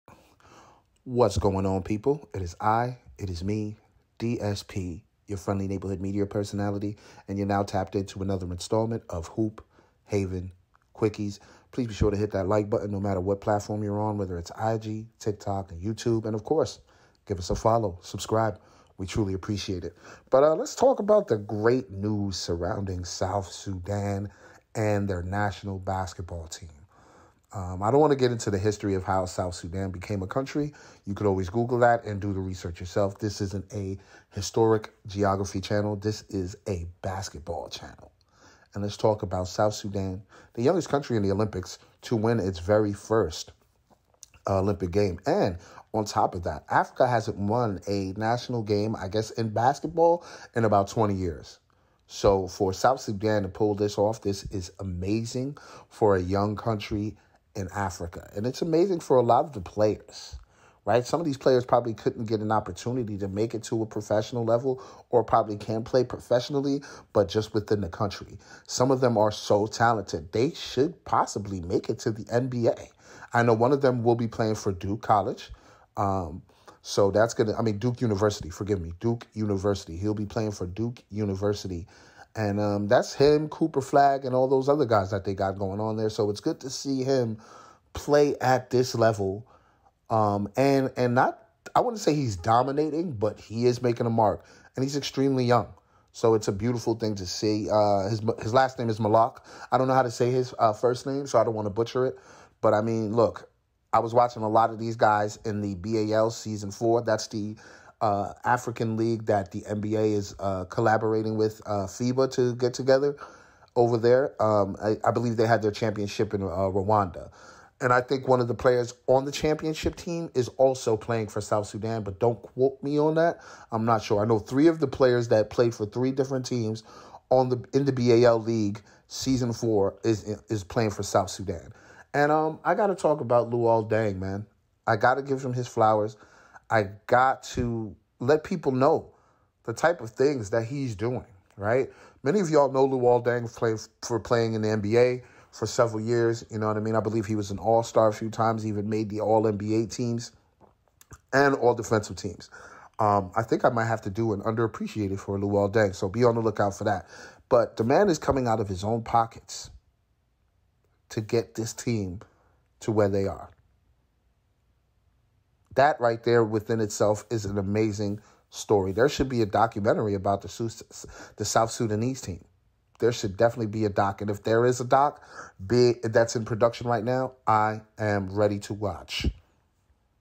Luol Deng & The South Sudanese Team Is Special
South Sudan are doing some incredible things! After dealing with flight delays among other nonsense, they came to the games & defeated Puerto Rico! All this because of the funding of Luol Deng, former NBA star, and this isn’t getting enough recognition!